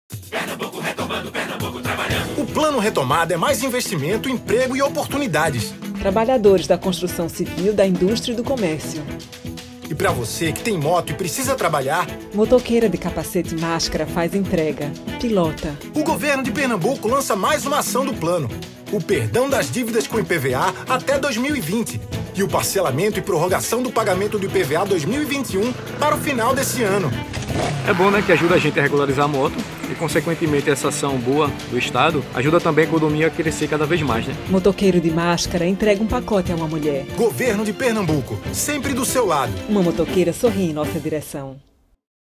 Motos: PE perdoa dívidas de IPVA, licenciamento e taxas estaduais anteriores a 2020 (AUDIODESCRIÇÃO)
PERNAMBUCO RETOMANDO 🛣🏍 // Para você que tem moto e precisa trabalhar, o Governo de Pernambuco concedeu o perdão das dívidas de IPVA, licenciamento e taxas estaduais, vencidos até 2020, para veículos de até 162 cilindradas. Além do parcelamento e da prorrogação do IPVA de 2021. As ações fazem parte do PlanoRetomadaPE, pacote de estímulos à geração de empregos e de incentivos à nossa economia.